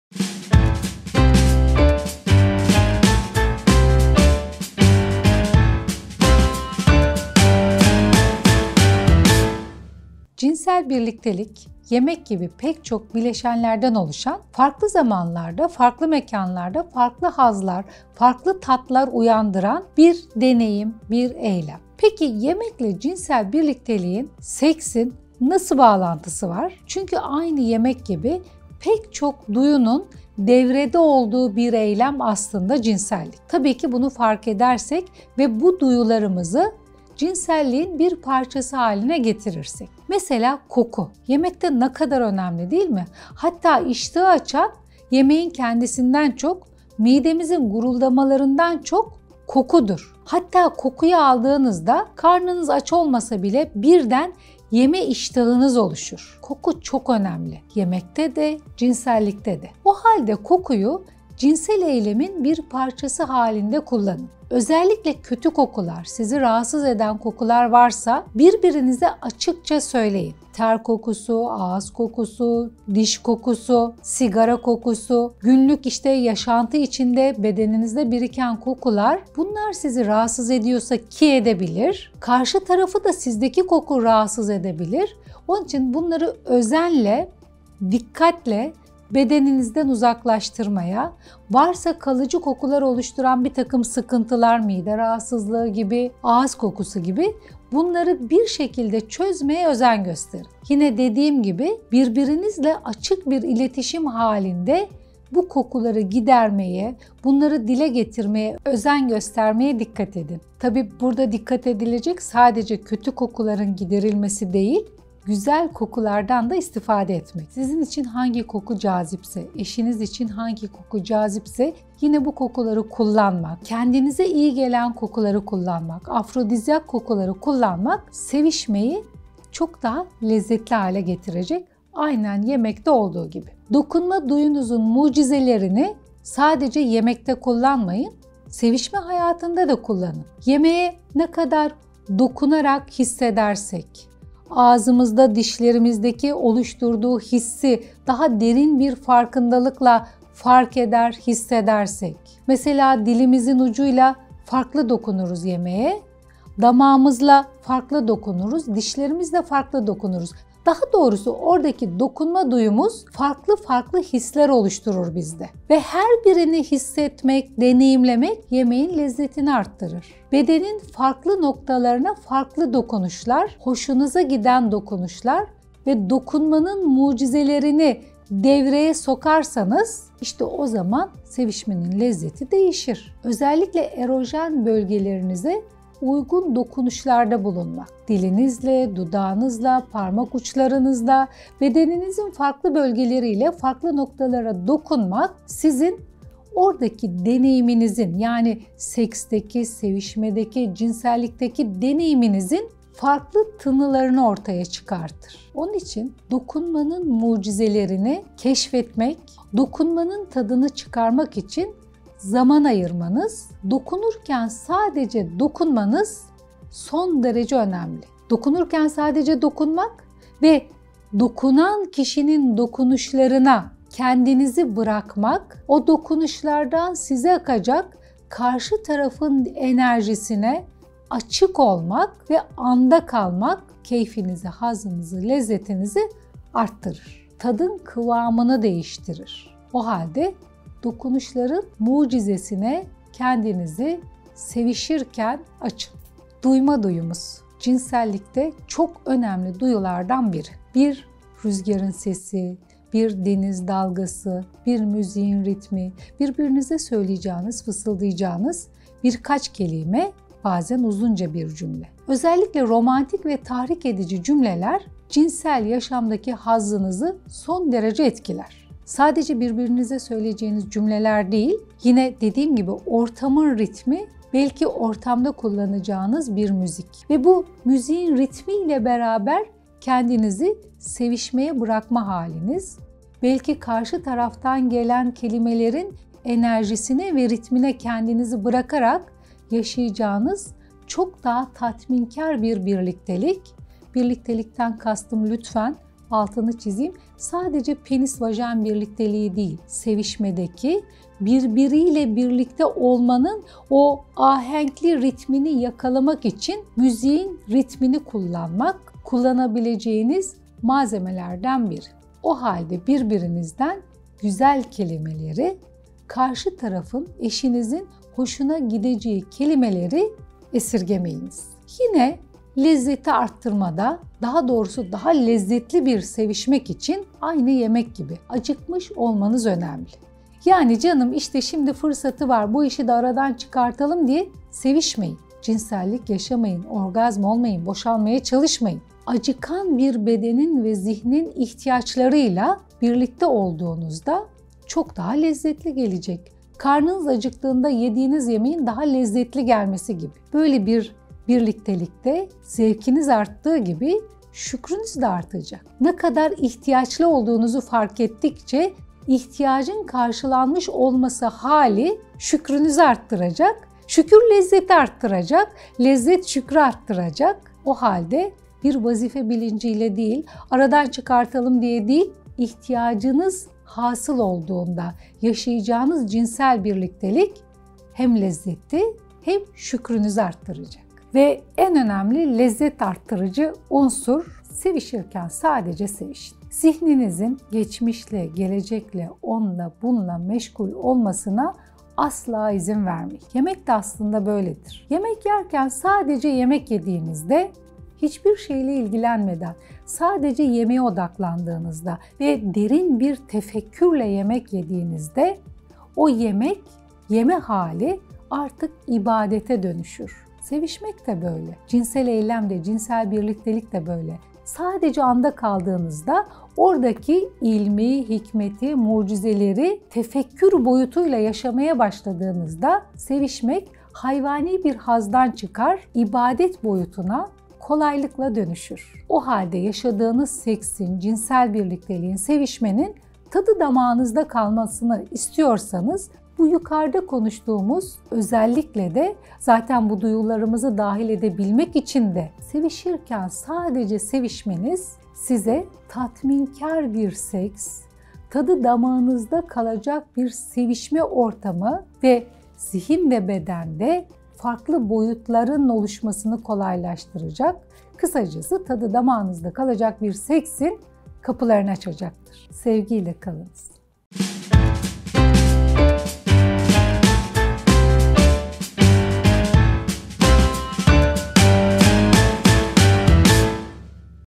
Tadı Damağınızda Seks
Op. Dr. Ayşe Duman'la Kadın Sağlığına Bütüncül Yaklaşım

Cinsel birliktelik; yemek gibi birçok bileşenlerden oluşan, farklı zamanlarda, farklı mekanlarda, farklı hazlar, farklı tatlar uyandıran bir deneyim, bir eylem.
Peki yemekle cinsel birlikteliğin nasıl bağlantısı var? Çünkü aslında aynı yemek gibi pek çok duyunun devrede olduğu bir eylem. Tabi ki bunu fark edersek ve bu duyularımızı cinselliğin bir parçası haline getirirsek. Mesela koku, yemekte ne kadar önemli değil mi?
Koku çok önemli, yemekte de cinsellikte de. O halde kokuyu cinsel eylemin bir parçası halinde kullanın. Özellikle kötü kokular, sizi rahatsız eden kokular varsa birbirinize açıkça söyleyin. Ter kokusu, ağız kokusu, diş kokusu, sigara kokusu gibi kokular sizi rahatsız ediyorsa ki edebilir. Karşı tarafı da sizdeki koku rahatsız edebilir. Onun için bunları özenle bedeninizden uzaklaştırmaya, bu tarz sıkıntılar varsa bu sıkıntıları çözmeye özen gösterin. Aynı zamanda güzel kokulardan da istifade edin.
Sizin ve eşiniz için hangi koku cazipse, yine bu kokuları, kendinize iyi gelen kokuları kullanmak sevişmeyi çok daha lezzetli hale getirir.

Sevgiyle kalınız efendim.

Instagram'dan da takip edebilirsiniz!

Özden Gelen Bütüncül Yaklaşımlarla Kadınlığın Yeniden Keşfi..